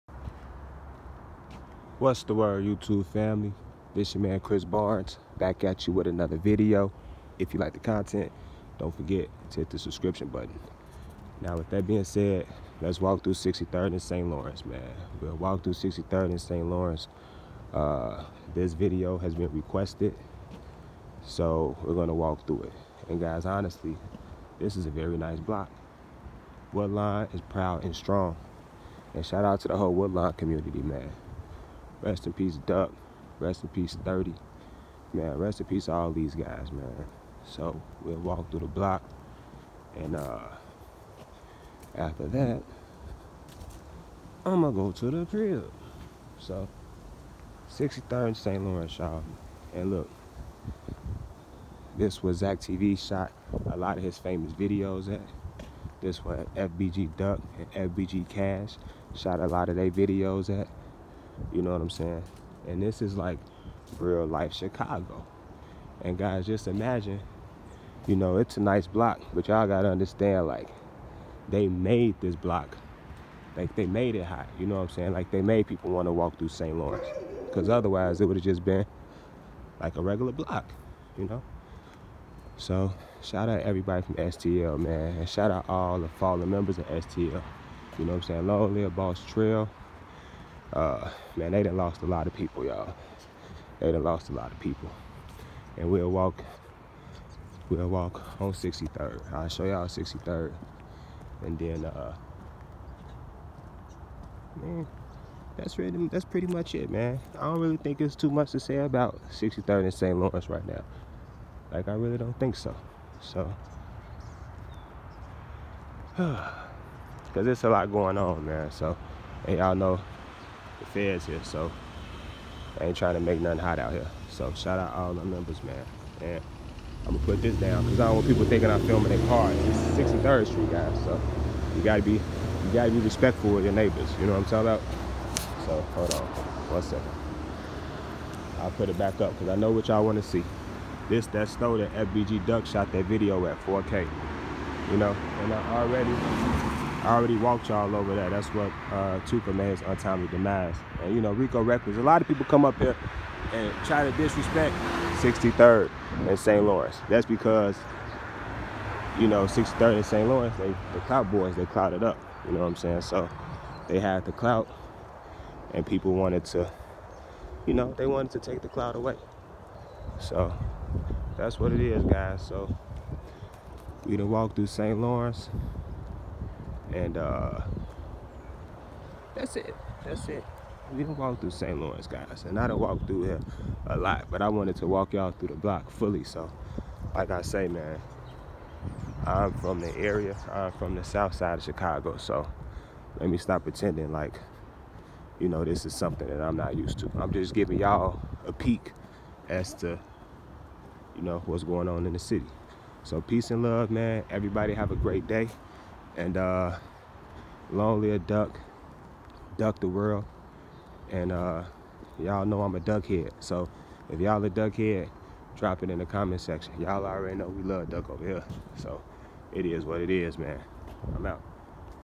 I Tried Walking Thru Tookaville and This is What Happened
Hey guys whatever you do dont be like me and try these walk thrus. these hoods are not safe and alot of bad things can happen if they dont know you. JUST WATCH